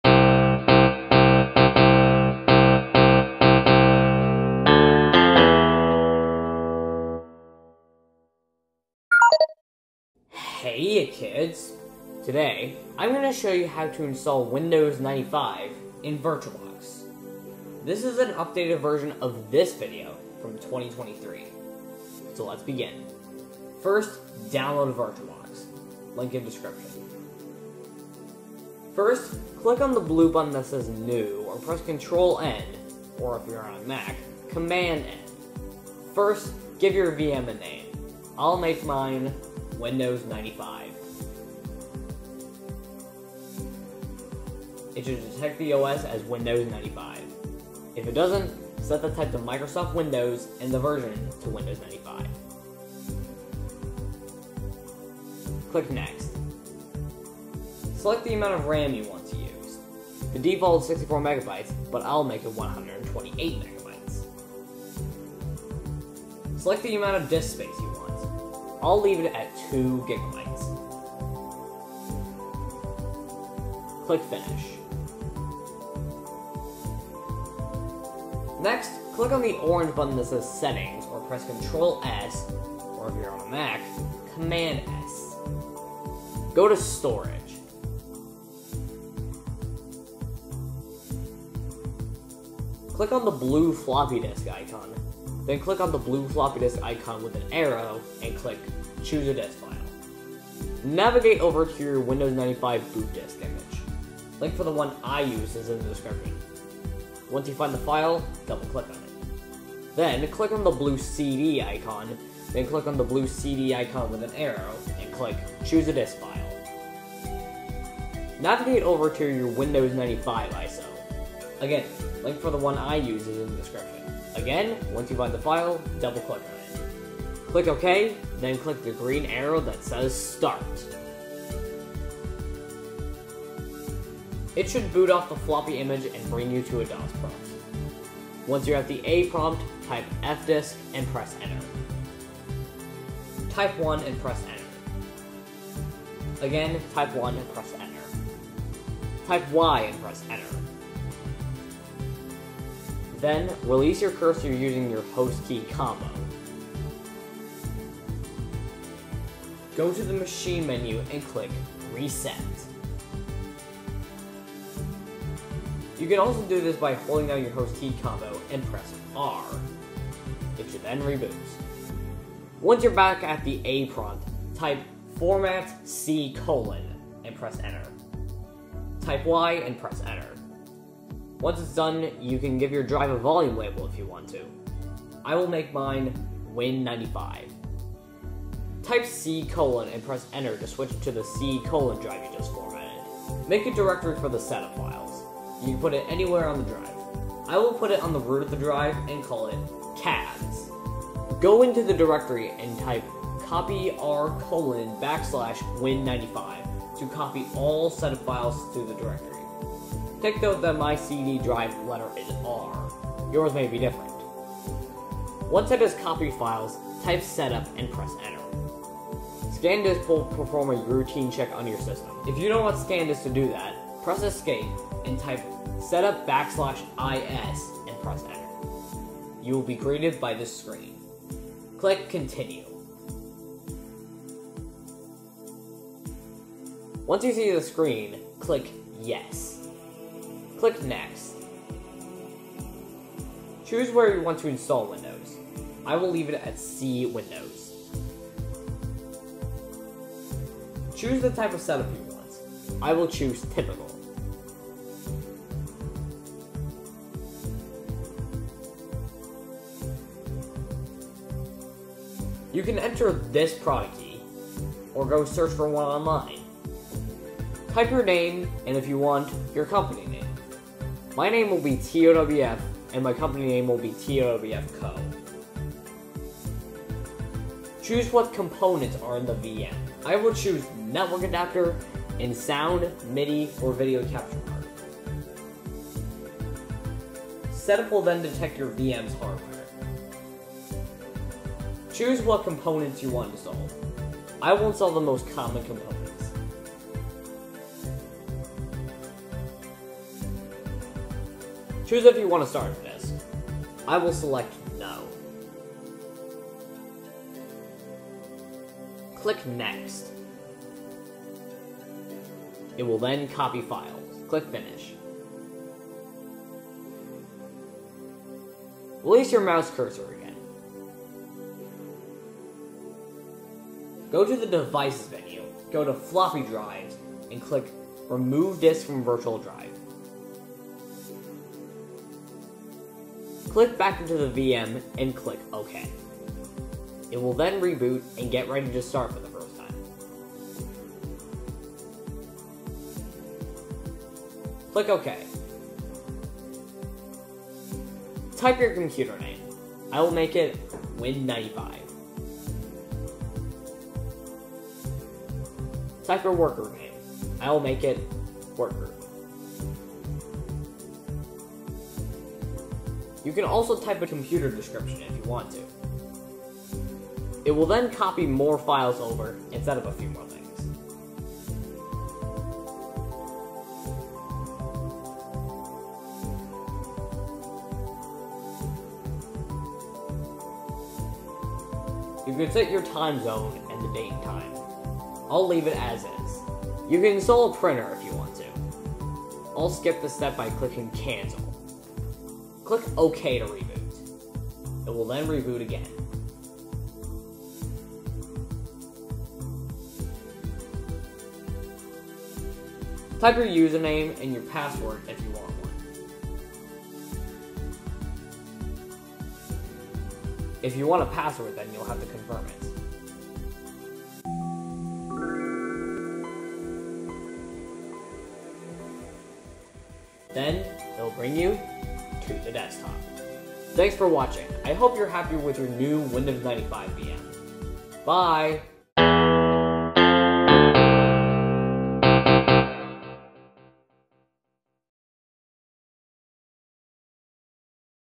How to install Windows 95 in VirtualBox (UPDATED 2025 VERSION)
This is an updated scripted version of "How to install Windows 95 in VirtualBox" from July 9, 2023.

I will NOT be reading comments. If you have a problem, go to the VirtualBox forums.

NOTE: I'm using VirtualBox 7.0 in this video. The process for 7.1 is a little different.

Follow me on Bluesky!